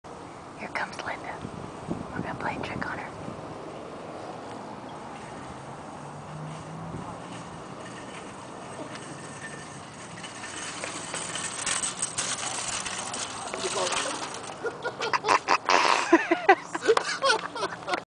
Puttin' a scare into Linda
Matt and I putting a good scare into Linda before she goes into Target.  I have a feeling she is going to get even.  For those who don't know her, she is the one who answers the phone at Label Technology.